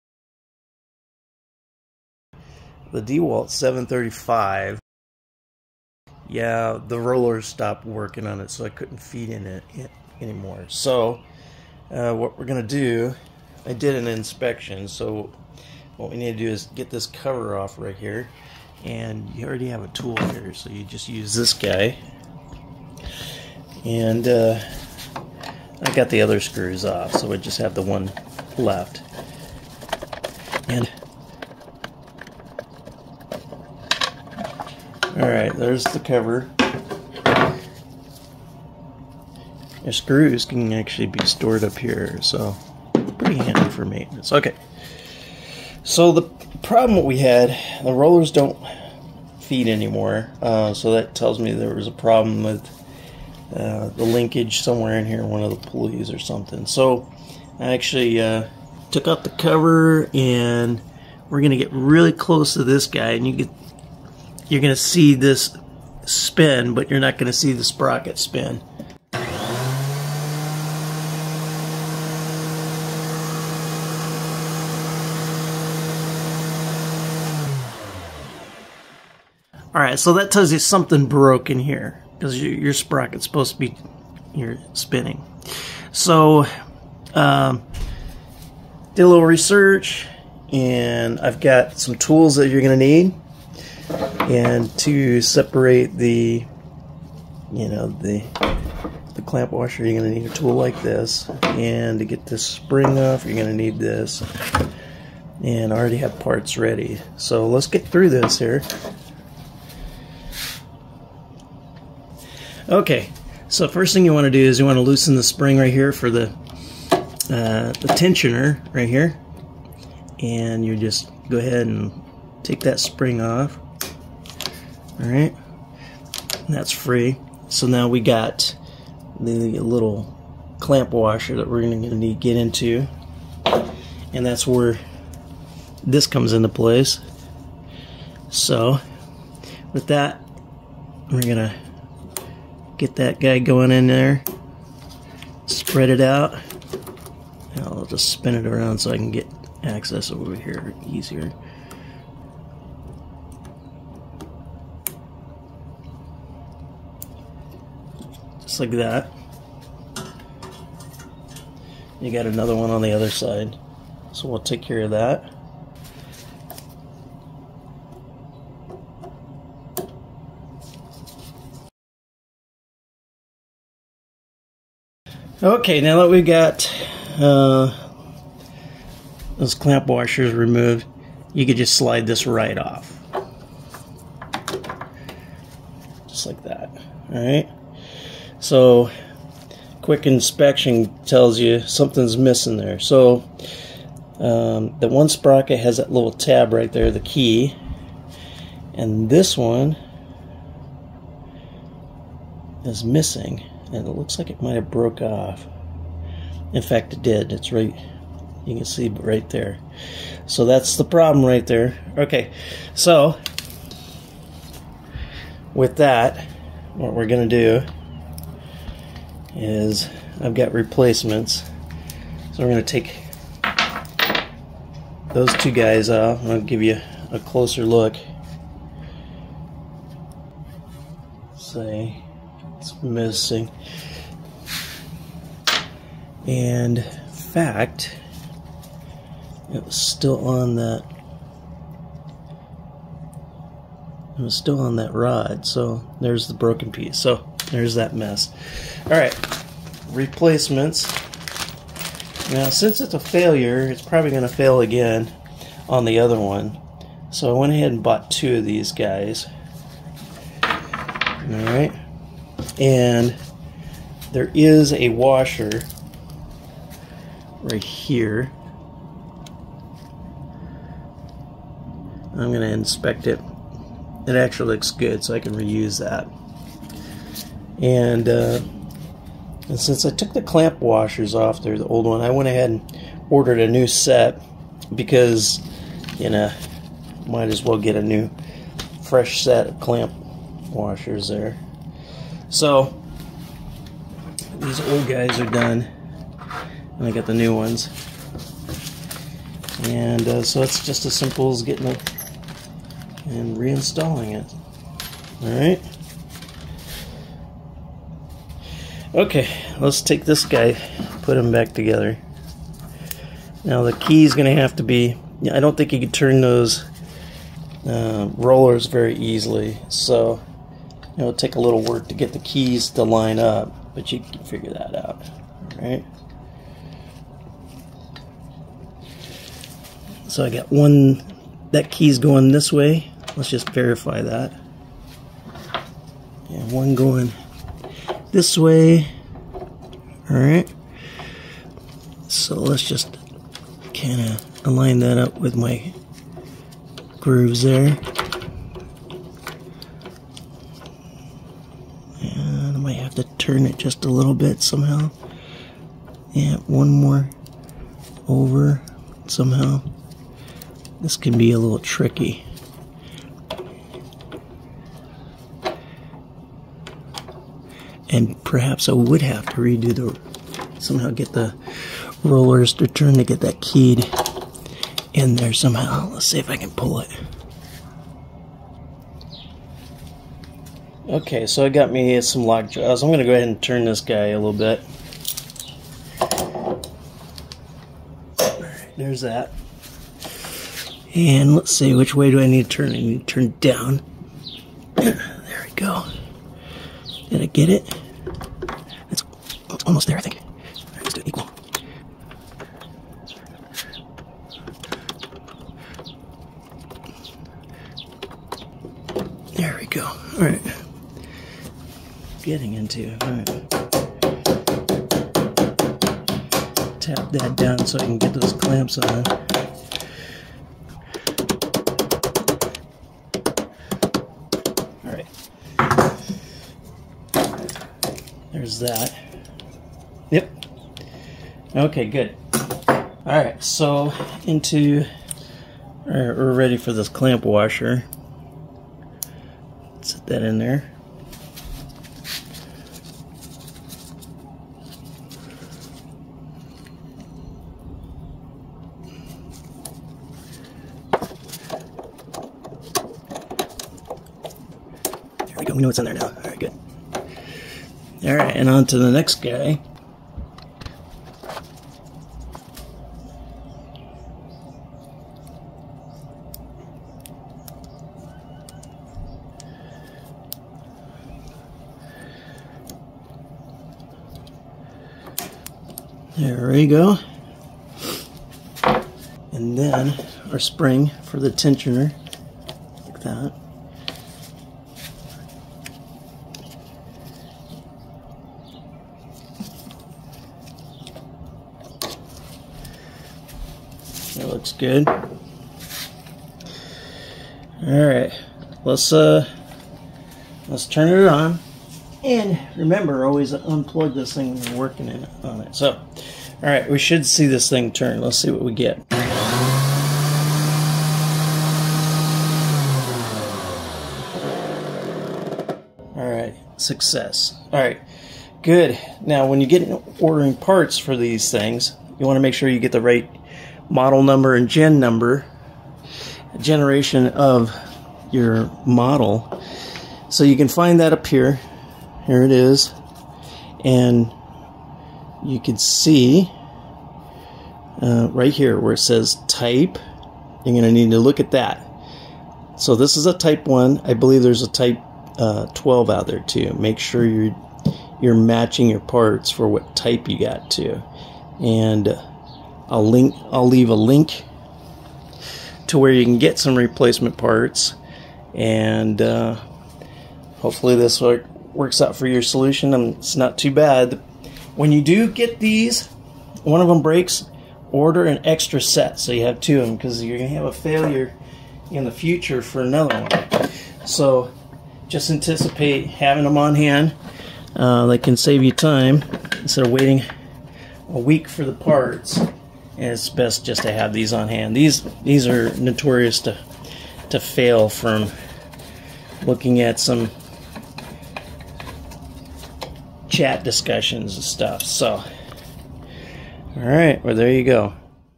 How to Fix DeWalt 735 Planar Feed Rollers not working.
the 'keyed' part of one of the feed roller sprockets broke off.

Where to find replacement parts-

DeWALT 13 inch Planer Replacement Parts For Model DW735 Type 1 @

Chapters:

0:00 - Intro- problem
0:13 - Inspection
0:44 - the problem
1:25 - tools and parts
1:49 - remove parts
3:06 - inspect bad part
4:37 - new parts
5:09 - install new parts
8:14 - test the new parts
8:38 - check correct model type/generation # for ordering parts